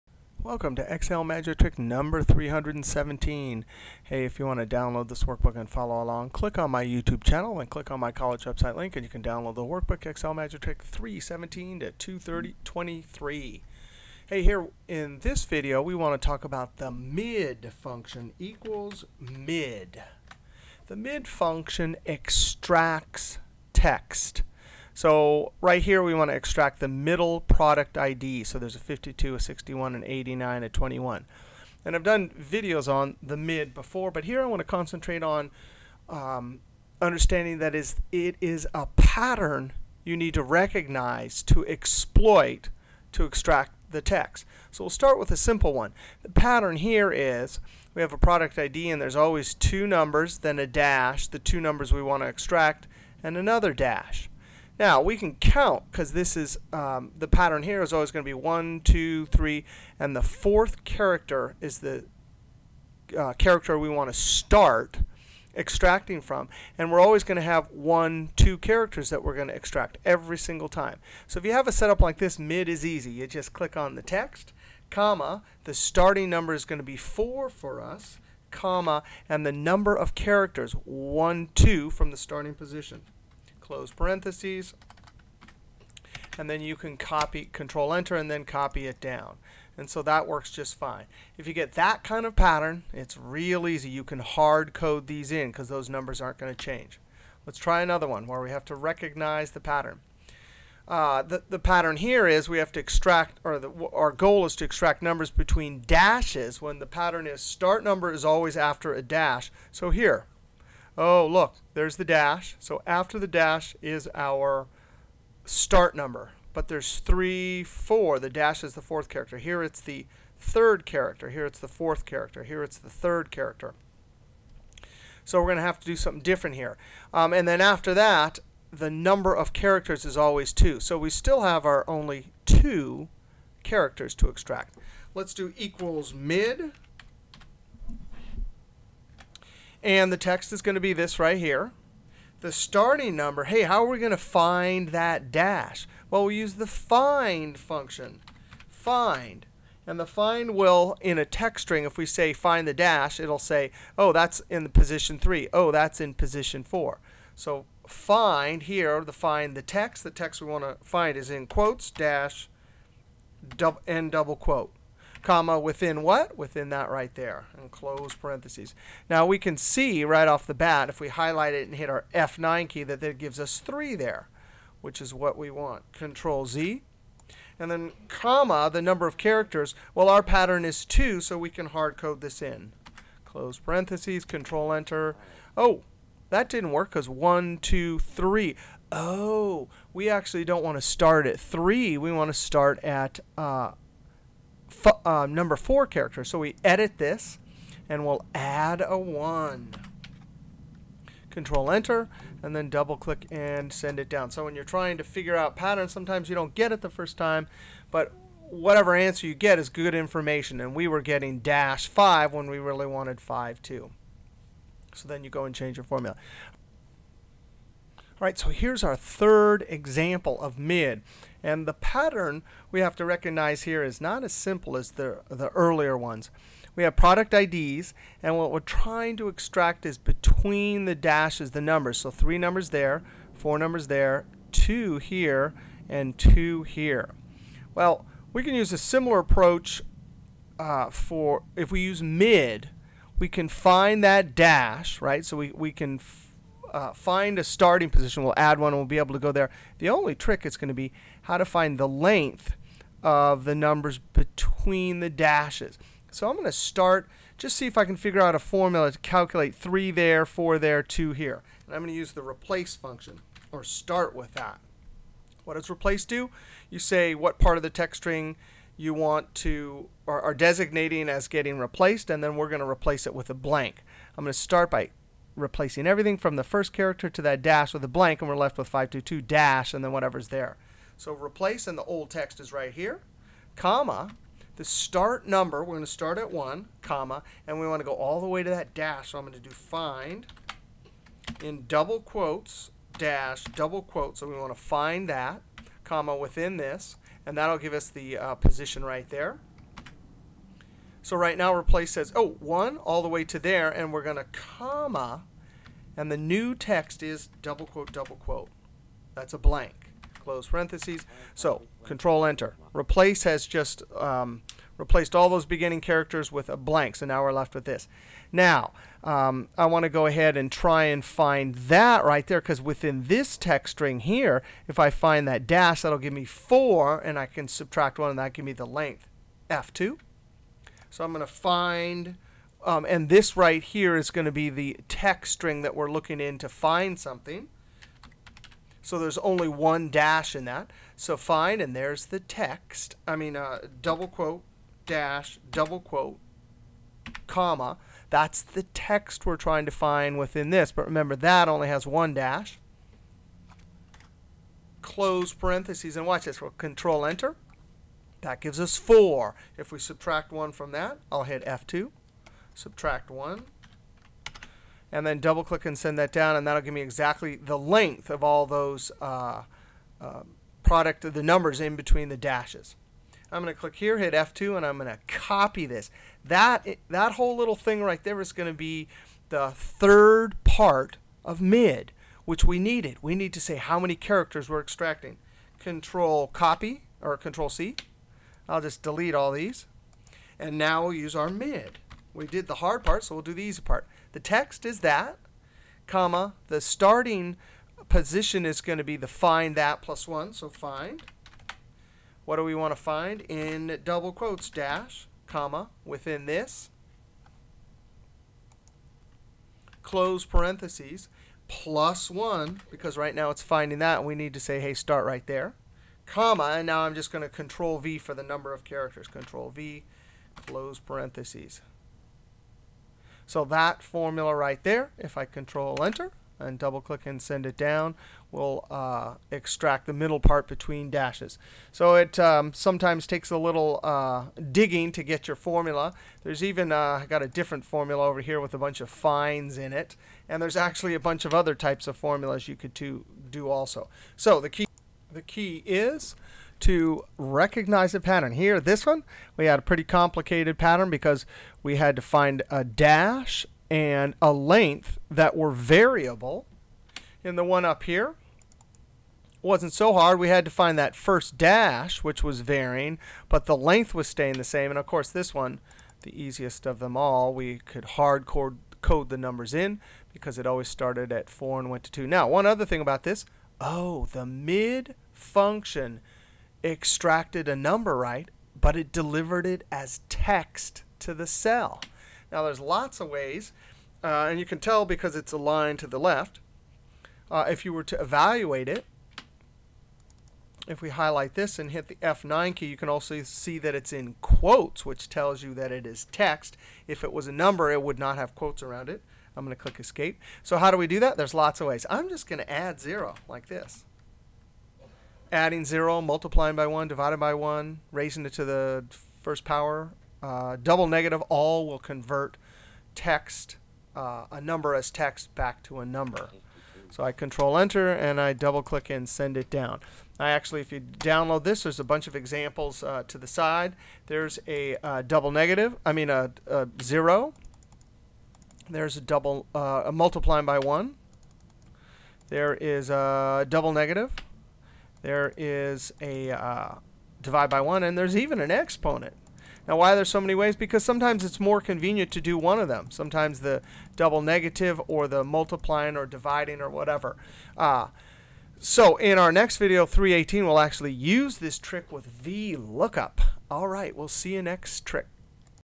Excel Magic Trick 317: MID function: Pattern Recognition
See how to Extract Data (substring from string) using the MID Function. They key formula concept is: Pattern Recognition. See 3 Examples.